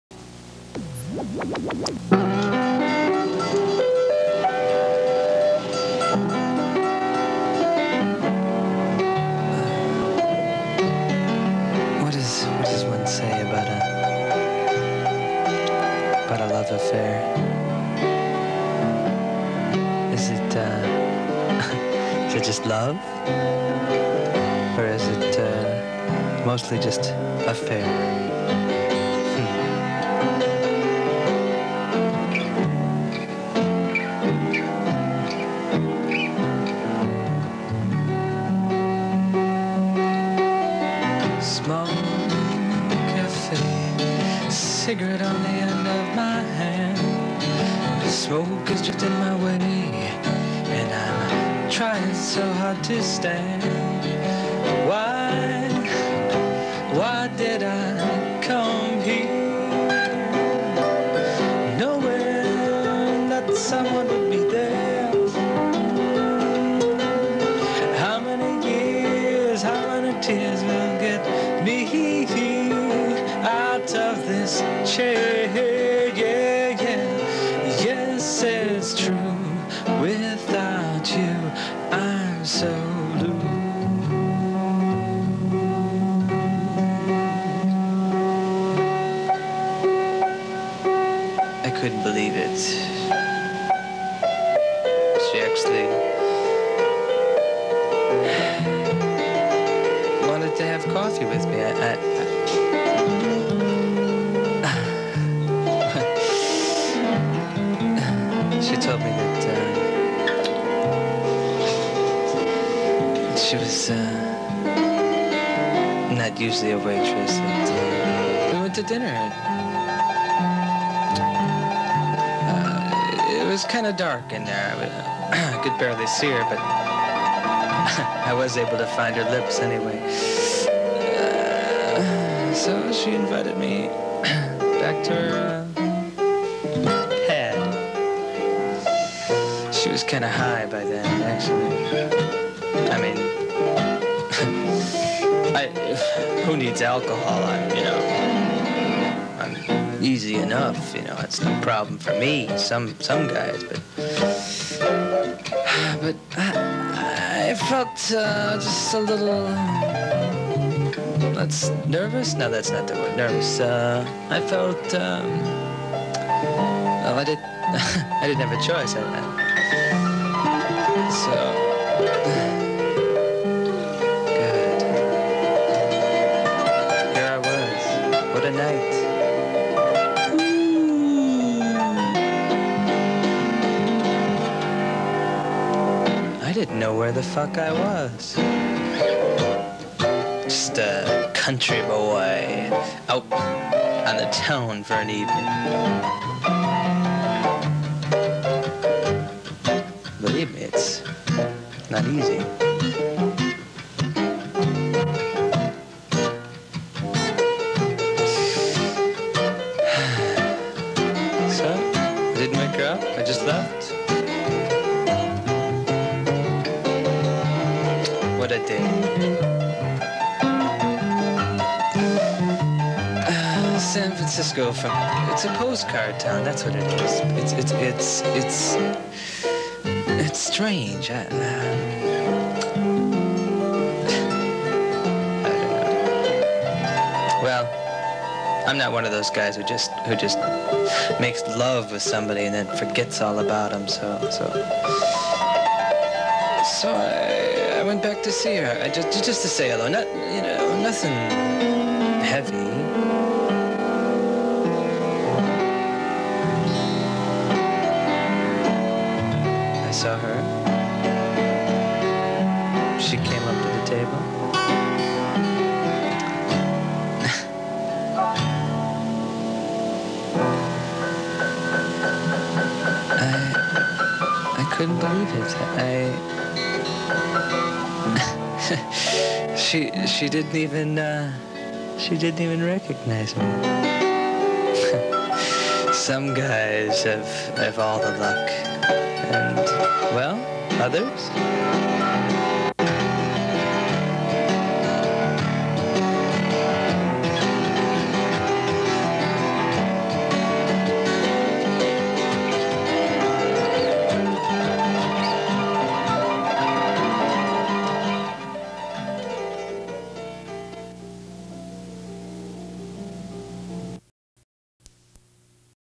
The San Franciscan student film
Film I made at USC with Marian Benassi and Richard Perry on super 8.